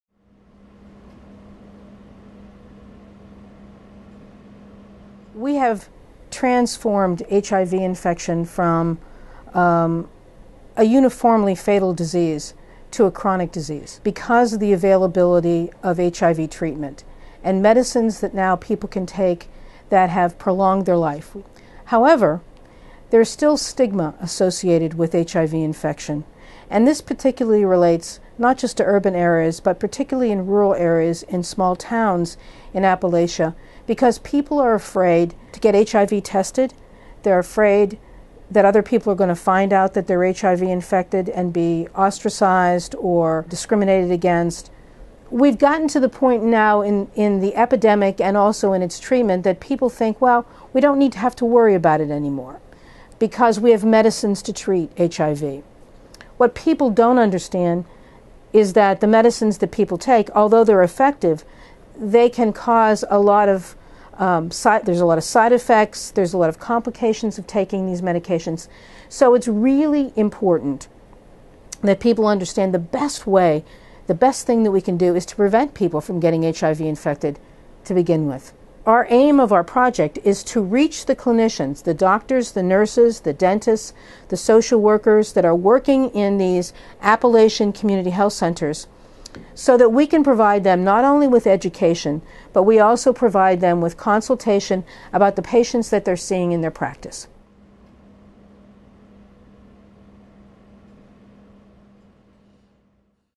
Telehealth Appalachian AIDS Education and Training Center Project
The University of Pittsburgh Graduate School of PublicHealth (GSPH), Department of Infectious Diseases and Microbiology is administering the Telehealth Appalachian AIDS Education and Training Center Project (TAAP).   TAAP provides web-based HIV education, training, clinical guidelines and resources to Appalachian community health centers.

The project is led by Linda Rose Frank, Ph.D., M.S.N., an associate professor of public health, medicine and nursing at the school, who has worked to increase knowledge and awareness about HIV disease in underserved communities in the hope of preventing discrimination and delivering more care to those who need it.

The research is funded by HRSA, Bureau of HIV/AIDS H4AHA22758.